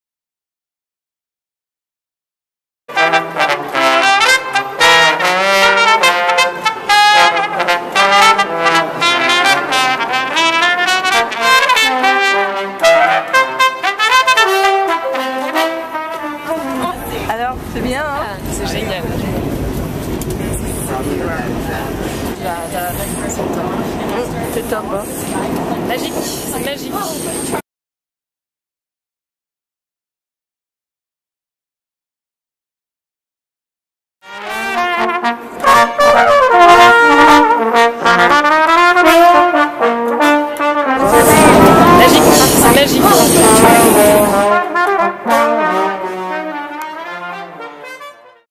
Aurel et Elo tribute, victimes of Paris Bataclan massacre
Aurelie de Peretti and Elodie Sappy, friends of mine from St. Tropez, France, visited me in NYC in November 2013. I showed them around the city and they especially enjoyed the subway music we found along the way.

Aurel and Elo were in Paris on November 13, 2015 attending a concert of Eagles Of Death Metal at the Bataclan concert hall. Aurel was killed, and Elo was gravely wounded. As of this post she is in a Parisian hospital.  My heart goes out to the families of these dear women. They loved life and music.

May the death of Aurel and the injuries of Elo not be turned into fear. I found this clip in my archives and am so happy to see them once again filled with light, love and wonderment.  As Aurel exclaims after hearing the brass duo perform on the platform at Broadway and Lafayette, C’est magique which needs no translation.

Love and Light to us all.  I was lucky enough to have know Aurel for the short time I did, and I hope one day to visit Elo and give her a huge hug of love and support.

Music heals.